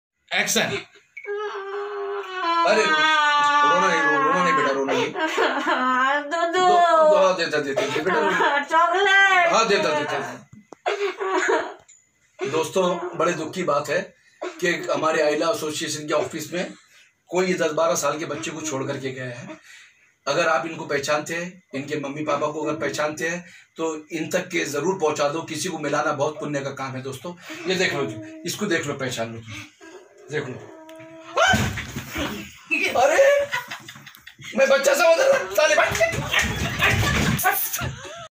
AILAA office में हुआ अजीब क़िस्सा हसी नहीं रुकेगी with Arif Khan Look Alike Anil Kapoor
Promo don't forget to watching full episode
Coming soon full episode

अजीब क़िस्सा हसी नहीं रुकेगी  with Arif Khan Look Alike of Anil Kapoor shorts video

Follow Tweeter.    :- JHAKAAS BOLLYWOOD